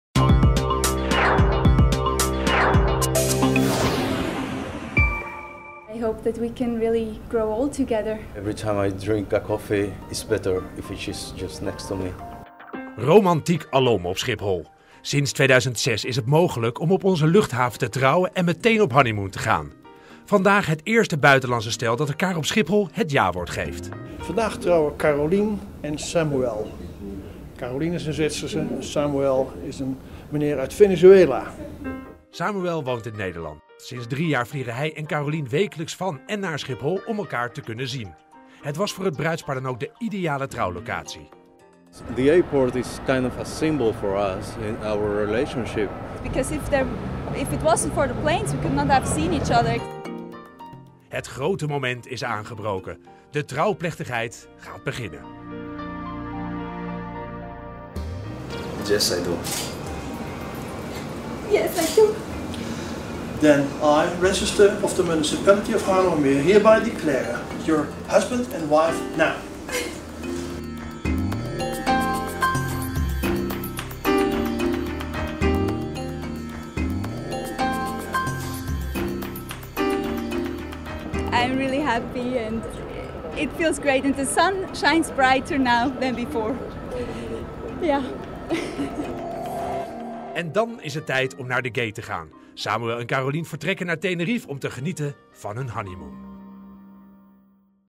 Eerste buitenlandse huwelijk op Schiphol
'Say yes & go!' Sinds 2006 zijn al honderden Nederlandse stellen op deze manier getrouwd op Schiphol.  Afgelopen week was het de beurt aan het eerste buitenlandse bruidspaar.